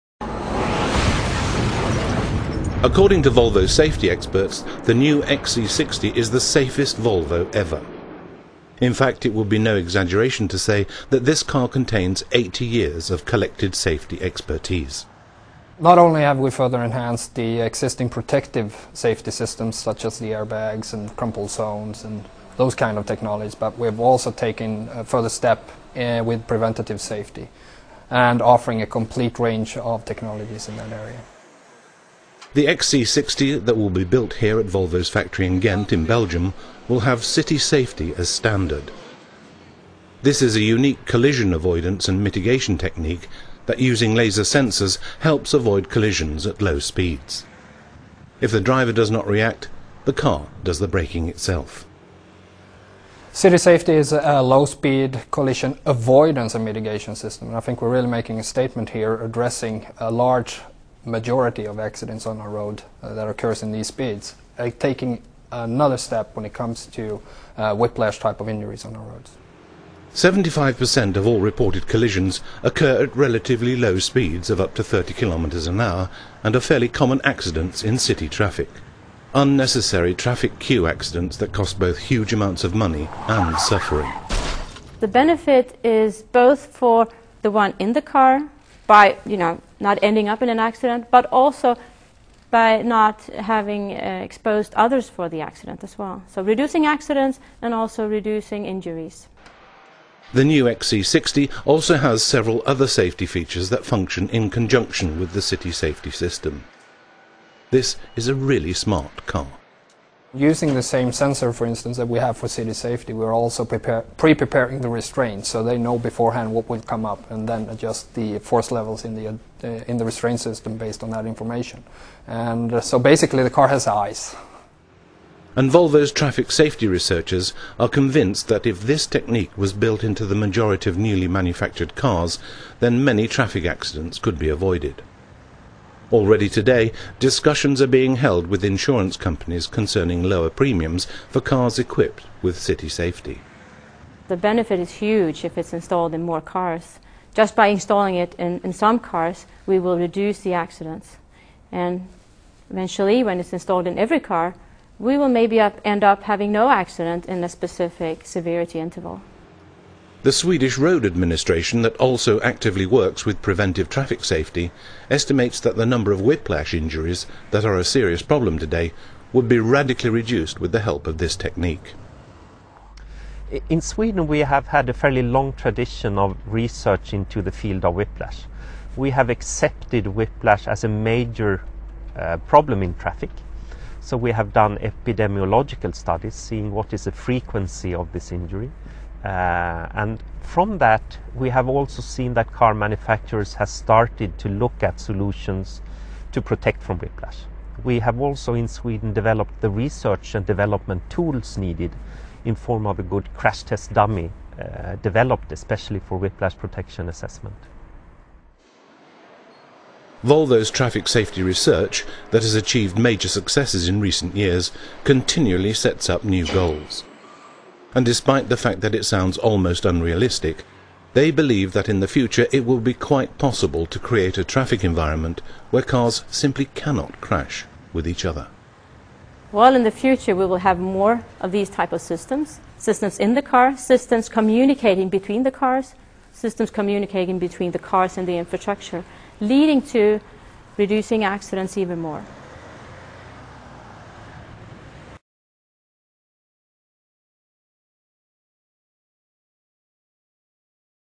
Volvo XC60 Safety System
A sort video on the various safety features in a XC60